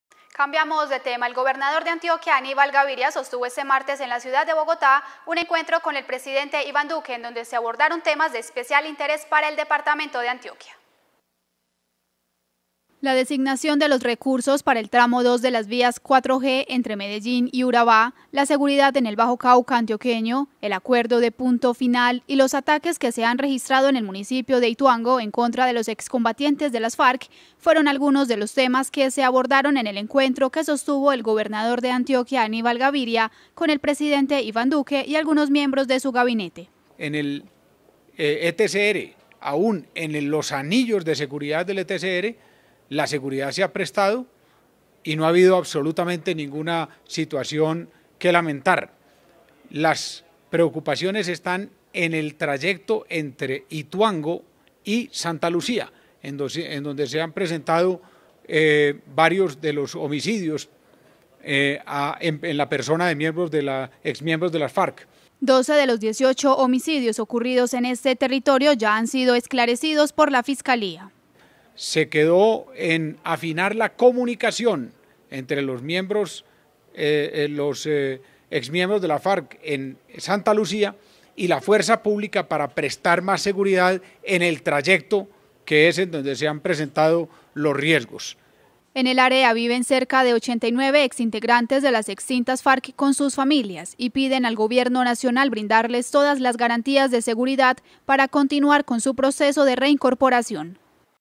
Encuentro entre el Gobernador de Antioquia, Aníbal Gaviria e Iván Duque.
El gobernador de Antioquia Aníbal Gaviria se reunió en Bogotá con el presidente Iván Duque para abordar temas de especial interés para el departamento de Antioquia.